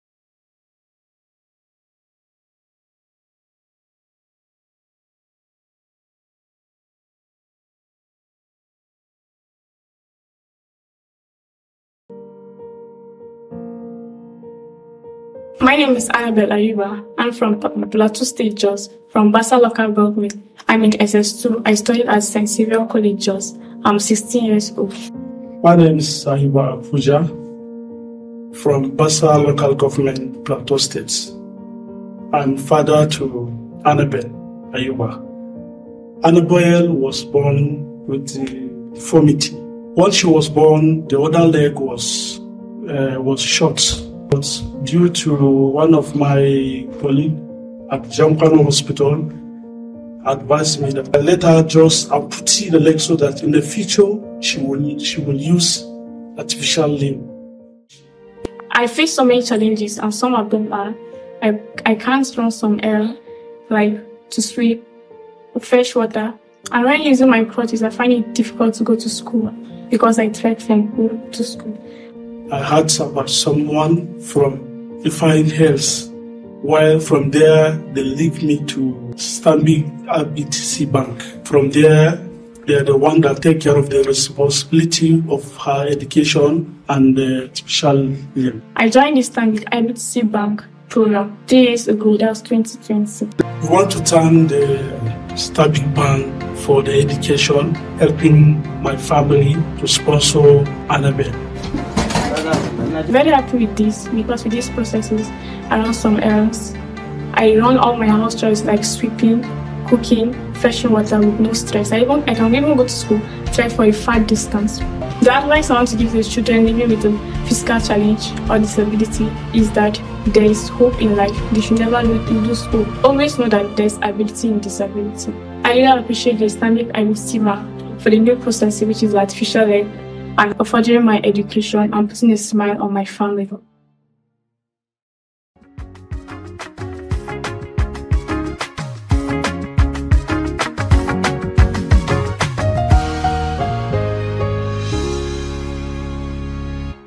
Anabel | A Day in The Life of a Beneficiary - Together4ALimb Series
Annabel has hope for a brighter future with her new prosthetic limb.
Watch a day in her life here.